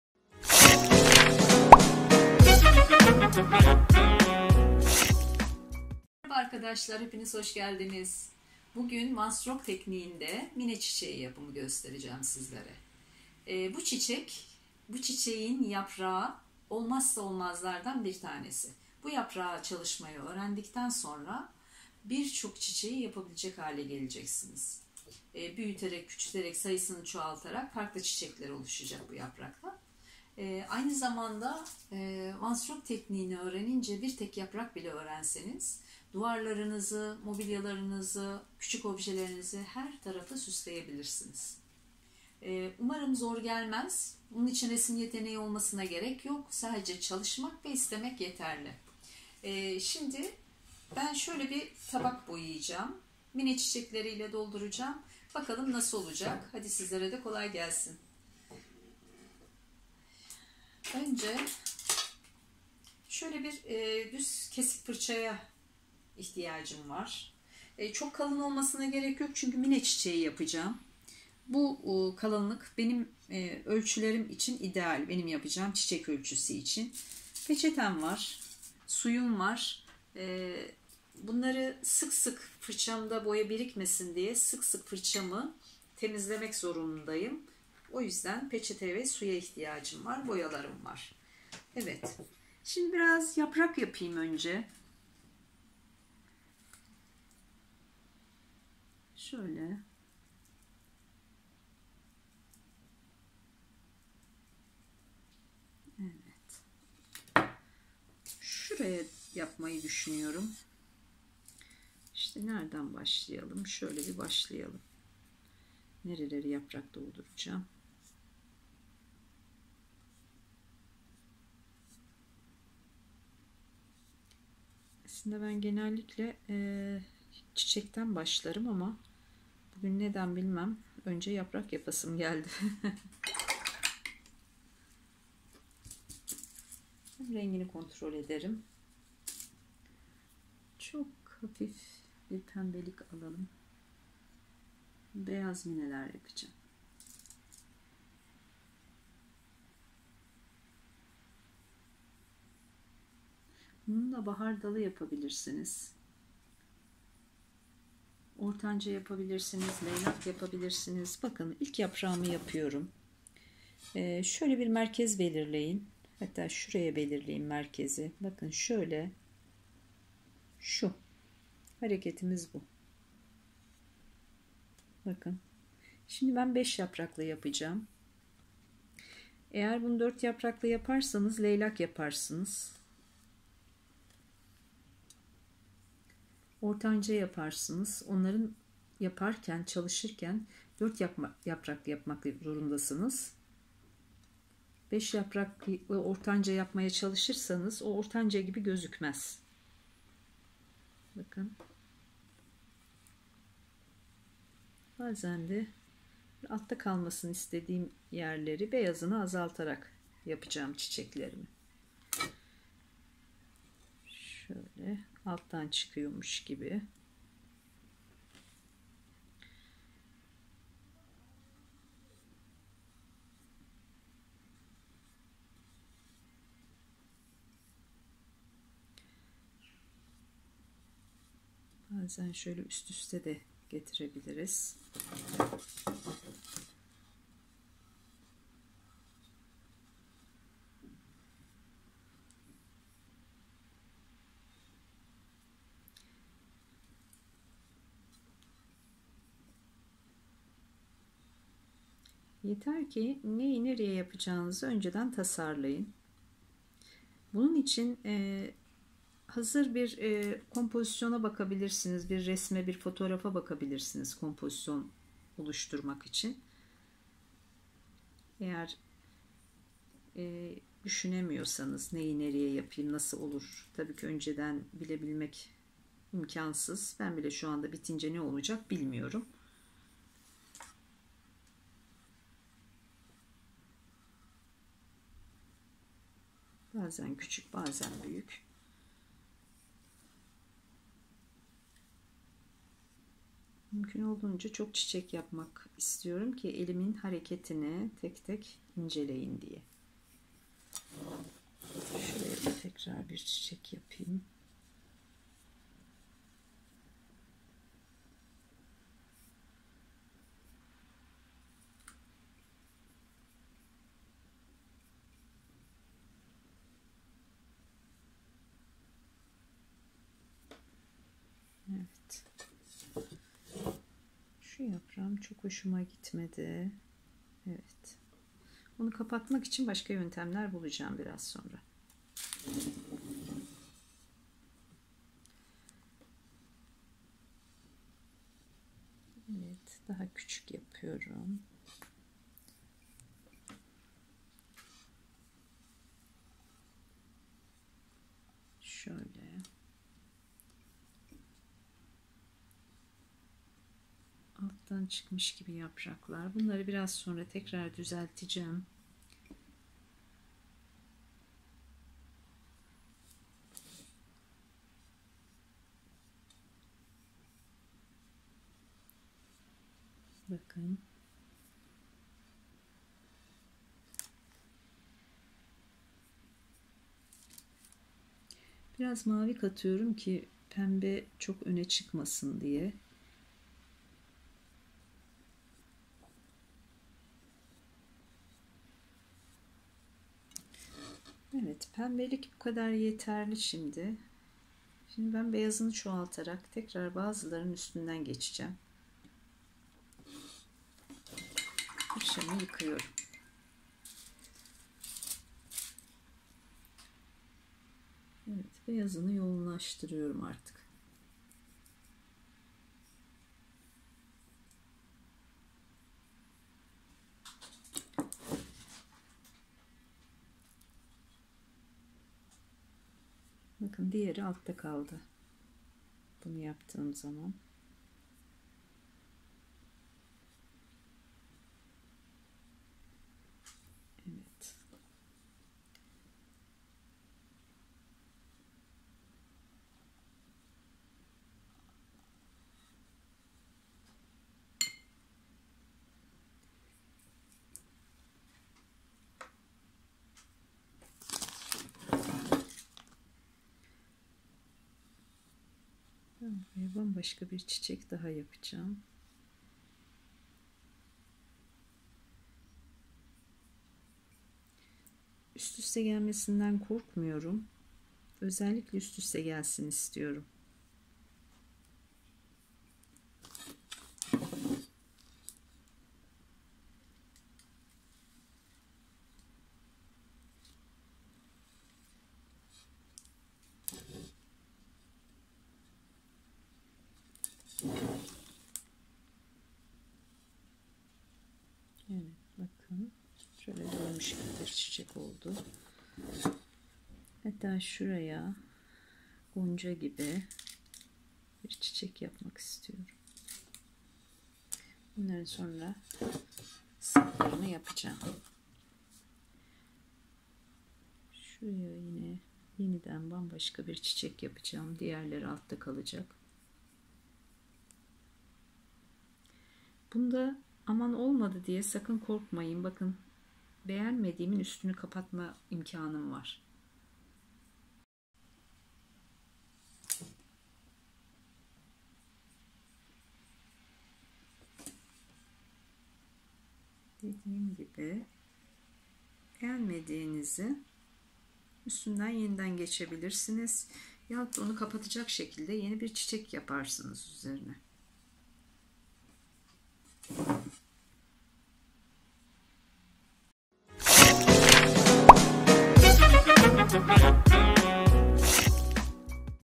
One Stroke Tekniği ile Basit Çiçek Yapımı ( MİNE ÇİÇEĞİ) / One Stroke Painting of Flowers
One Stroke tekniği çok zor gibi gözüken ama ufak yardımlar ve püf noktalarının öğrenilmesiyle herkesin 7 den 70 e zevkle çalışabileceği bir tekniktir.
     Bu teknikle hem resim bilginizi geliştirebilir hem de bu bilgiler ile evinizin her köşesinde ufak dokunuşlarla değişikliklere gidebilirsiniz. Tek ihtiyacınız olan şey HAYAL GÜCÜNÜZ

One Stroke Painting of flowers